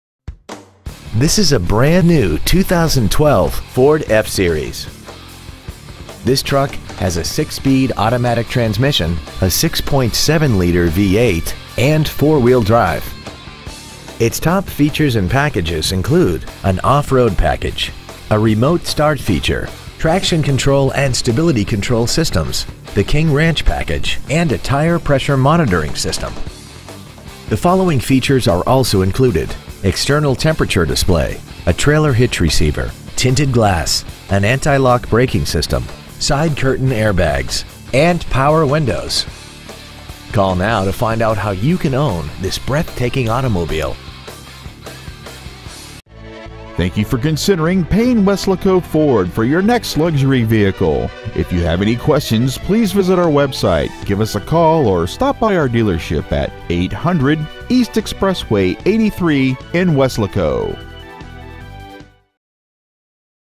2012 Ford F-350 Corpus Christi TX
Year : 2012
 Make : Ford
 Model : F-350
 Engine : 6.7L V8 Diesel
 Trans . : Automatic 6-Speed
 Exterior : Forest Grn

 Interior : Adobe w/Unique King Ranch Leather 40/Console/40 Fr
 Stock : EB00377

 800 E. Exp. 83